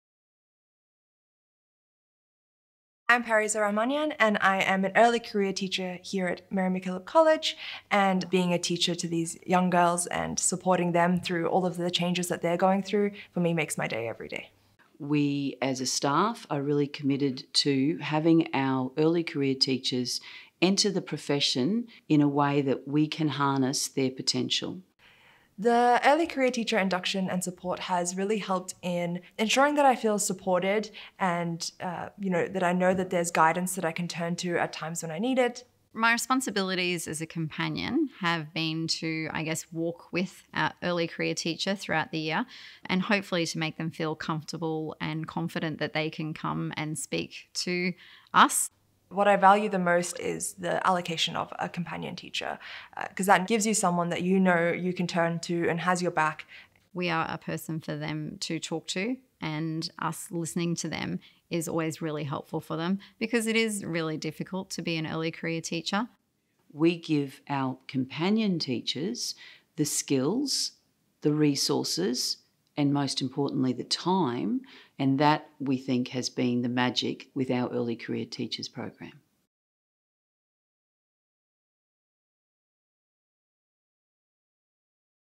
Pareesa's Story - Early Career Teacher
Hear from Pareesa and her journey to starting her teaching career at Mary MacKillop College, Nundah.